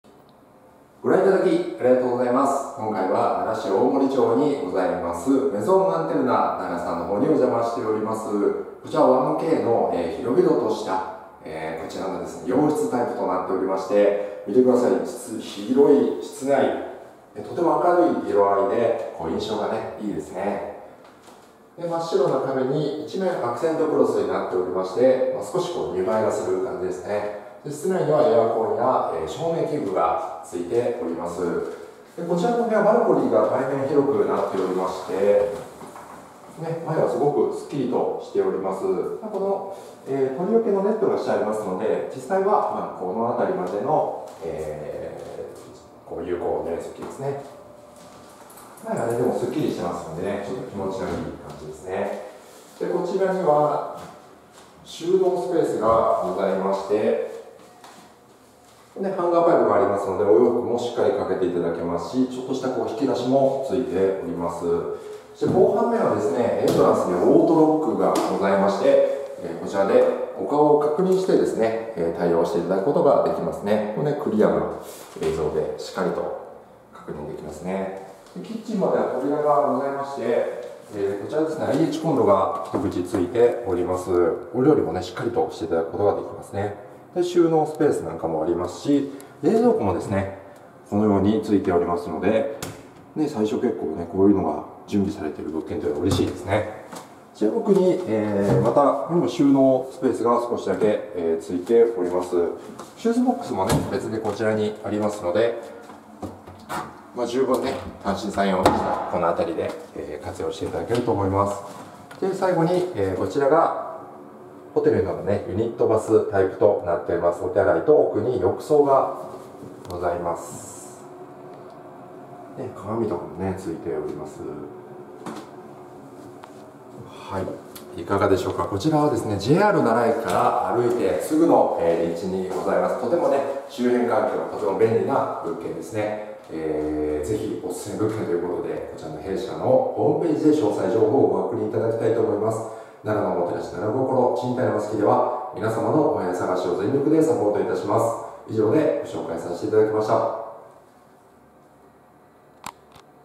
【ルームツアー】メゾンアンテルナ奈良｜奈良市奈良駅賃貸｜賃貸のマサキ｜Japanese Room Tour｜001280-1-7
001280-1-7
奈良県奈良市大森町24-1にあります評判の良い間取りの1Kマンション【メゾンアンテルナ奈良】の物件紹介動画です。